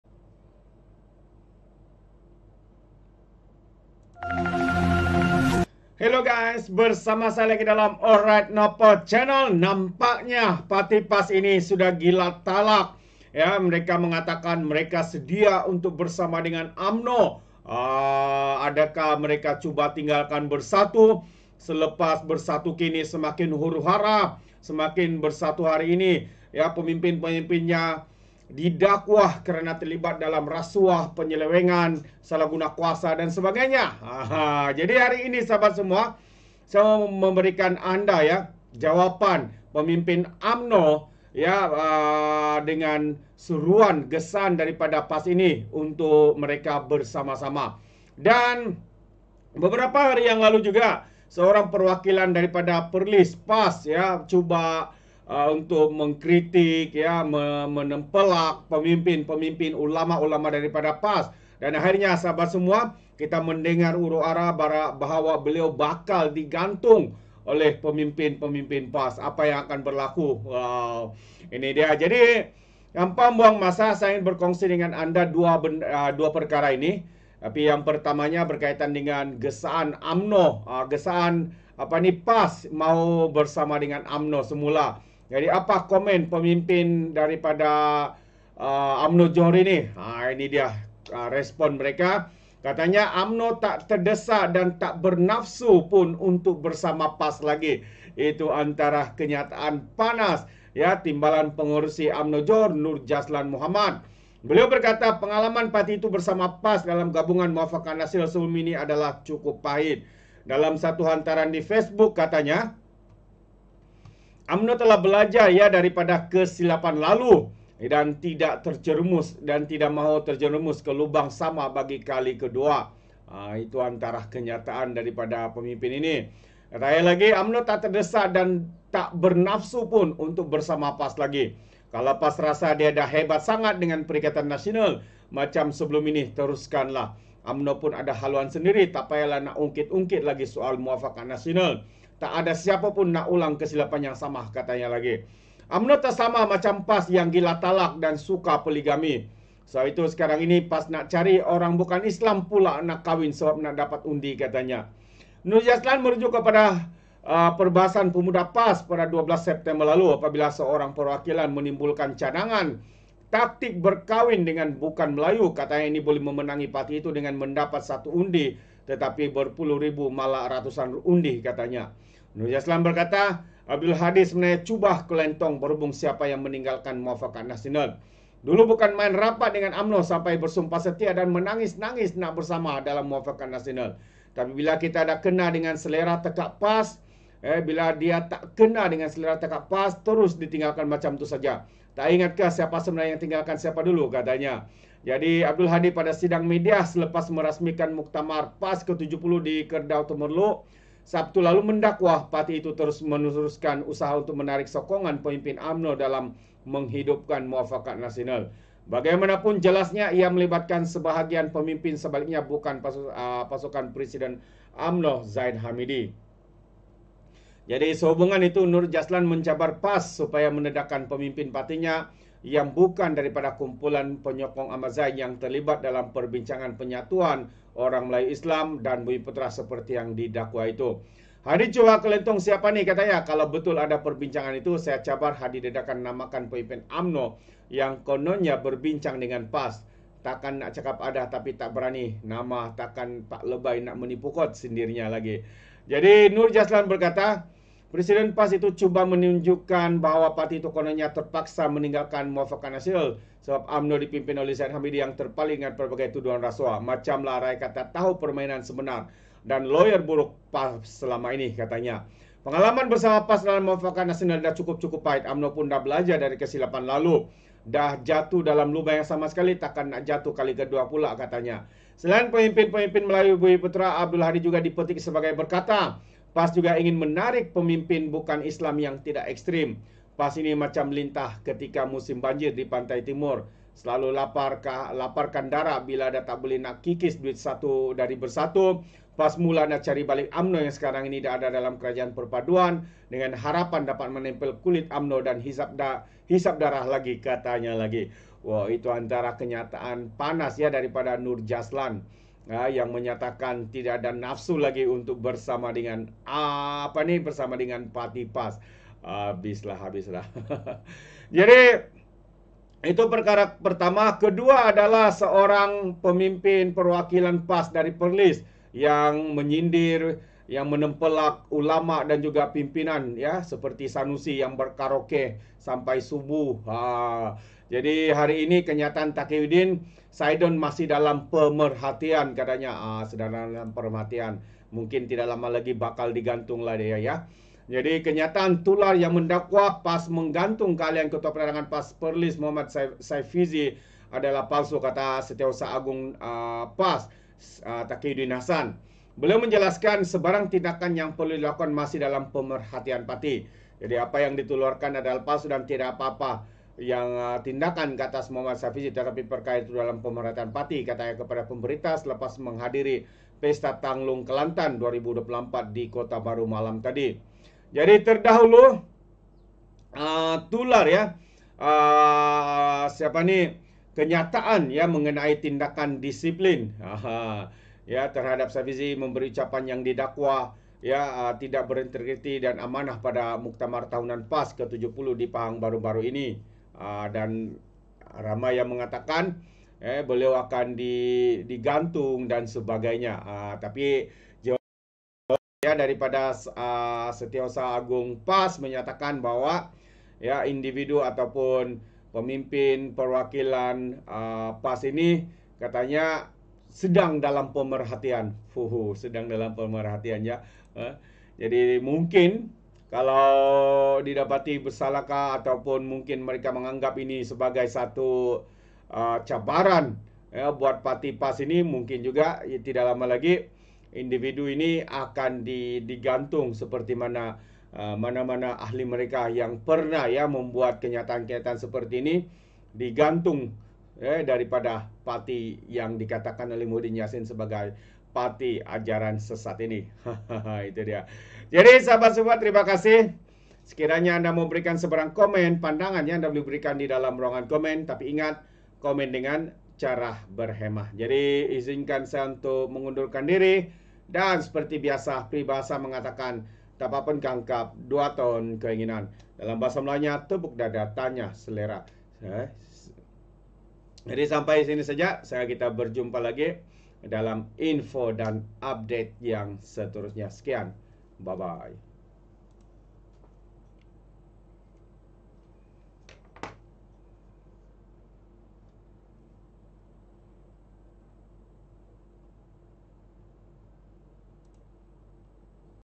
BAKAL DIGANTUNG KRITIK PEMIMPIN PAS! UMNO TIDAK TERDESAK BERSAMA PARTI PAS!
Dalam satu hantaran di Facebook, katanya, Umno telah belajar dari kesilapan lalu dan tidak akan terjerumus ke lubang sama bagi kali kedua.
“Umno tak terdesak dan tak bernafsu pun untuk bersama PAS lagi.
“Kalau PAS rasa dia dah hebat sangat dengan Perikatan Nasional (PN) macam sebelum ini, teruskanlah. Umno pun ada haluan sendiri. Tak payah la nak ungkit-ungkit lagi soal MN, tak ada sesiapa pun nak ulang kesilapan yang sama.
“Umno tak sama macam PAS yang gila talak dan suka poligami. Sebab itu sekarang ini orang PAS nak cari orang bukan Islam pula nak kahwin sebab nak dapat undi,” katanya.
Kenyataan tular yang mendakwa PAS menggantung keahlian Ketua Penerangan PAS Perlis Mohd Saifizi Saidon adalah palsu, kata Setiausaha Agung PAS Datuk Seri Takiyuddin Hassan.
Beliau menjelaskan sebarang tindakan yang perlu dilakukan masih dalam pemerhatian parti.
"Apa yang ditularkan adalah palsu dan tiada apa-apa tindakan ke atas Mohd Saifizi tetapi perkara itu dalam pemerhatian parti," katanya kepada pemberita selepas menghadiri Pesta Tanglung Kelantan 2024, di Kota Bharu malam tadi.